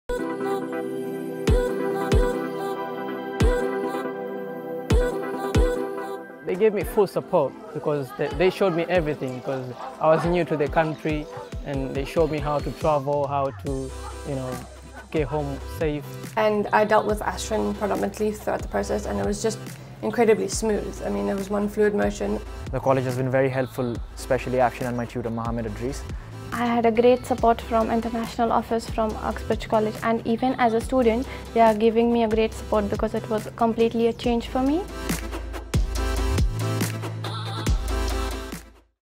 Harrow College & Uxbridge College (HCUC) Application Process for International Students
Harrow College & Uxbridge College (HCUC)  welcomes International Students. Here some of our students talk about the application process and what they like about studying with us.